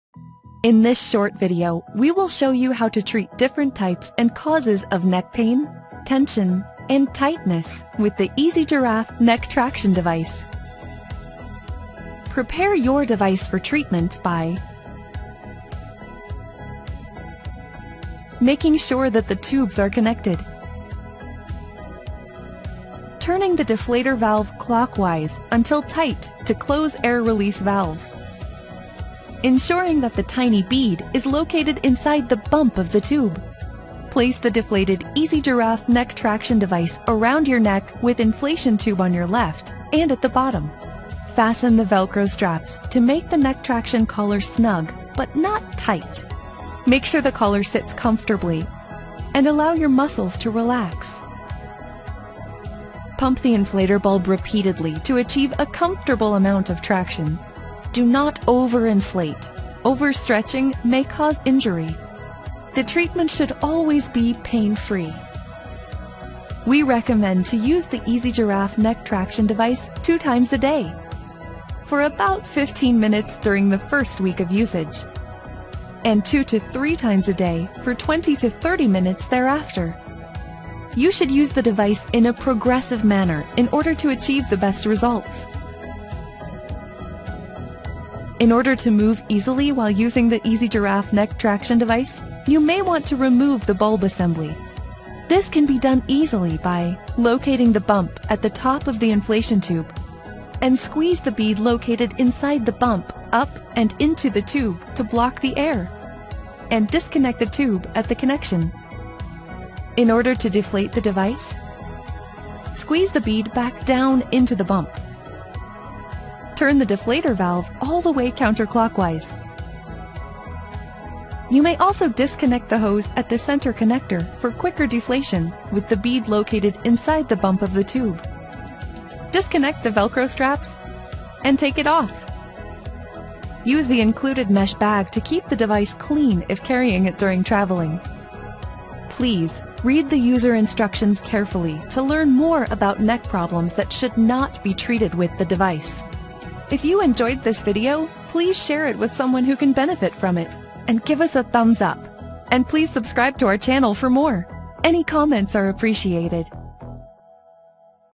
Portable Cervical Traction Device for Neck Pain Relief and Physical Therapy.
The Original Neck Hammock is a patented cervical traction assistant that works by gently pulling the head away from the neck to stretch and decompress the spinal cord, which increases bloodflow, oxegenates the surrounding muscles, decreases pain, and increases mobility of the neck and shoulders.